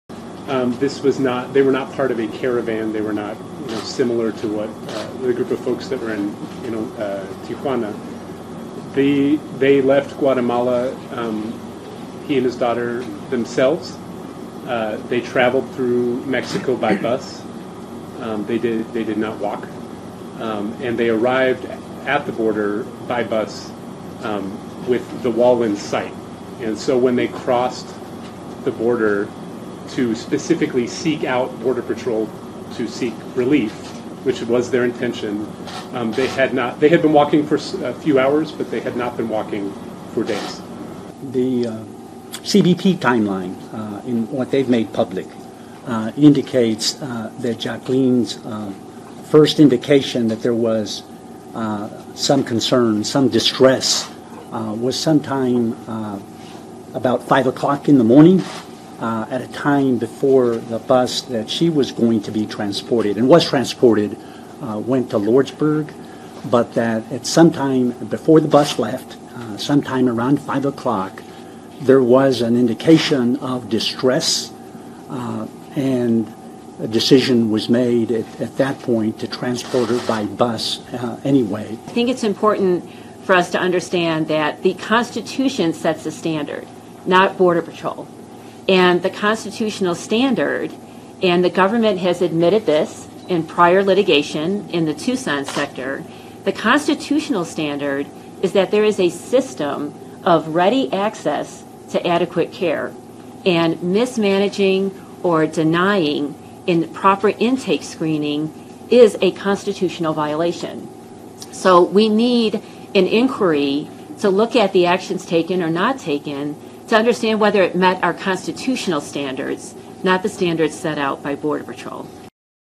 Family Lawyers Want Review Of Child Border Death
December 19, 2018 - Attorneys representing the family of 7-year-old Guatemalan girl who died in the custody of Border Patrol want and independent review of what led to the child's death. (Dec. 19)